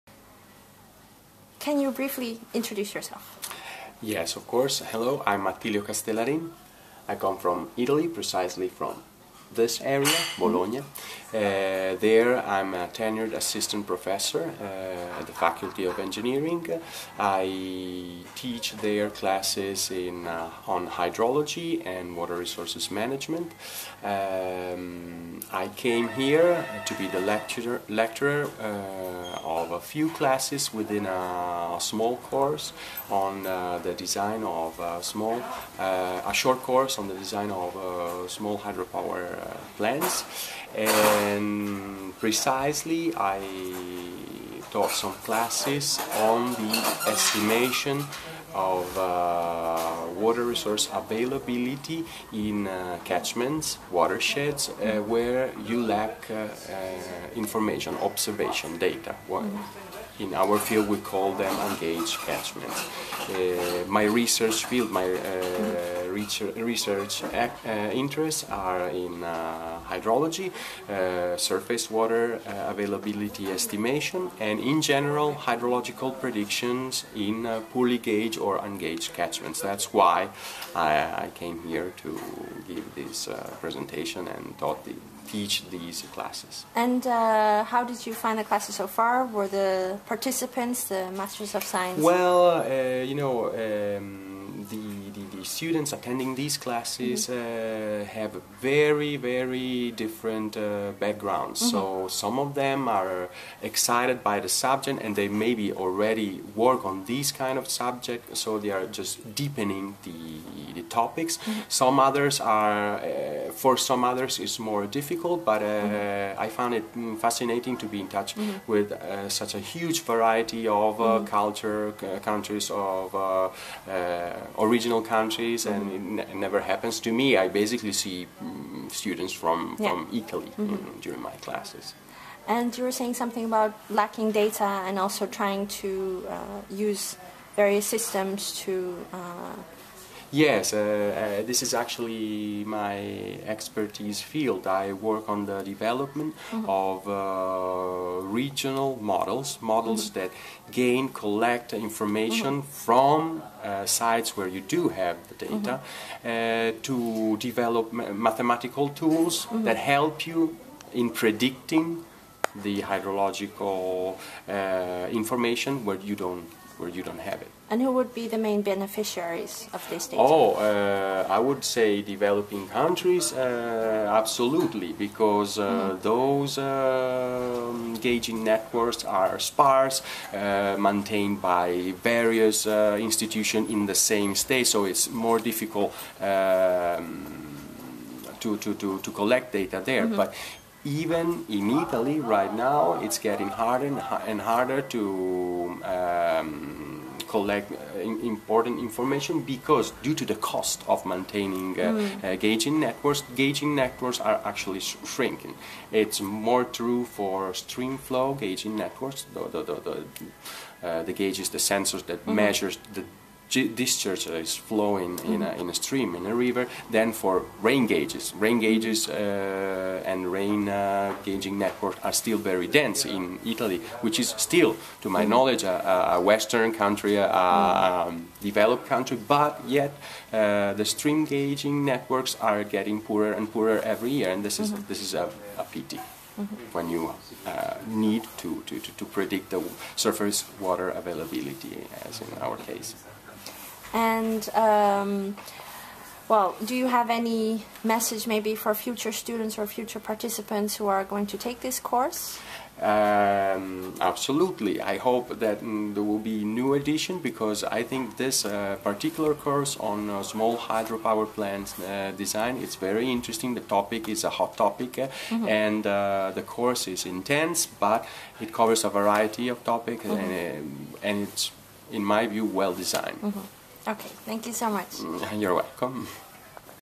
Attilio Castellarin University of Bologna
Dr. Castellarin, Associate Professor at the University of Bologna, Italy visited the Institute as a guest lecturer and gave a lunch seminar on smooth regional estimation of streamflow indices (canonical-kriging and top-kriging). His main research interests focus on anthropogenic effects on hydrological processes; predictions in ungauged basins, regionalisation of hydrological information, catchment classification; GIS hydrological applications; numerical one- and two-dimensional hydrodynamic modelling; flood risk assessment and mapping.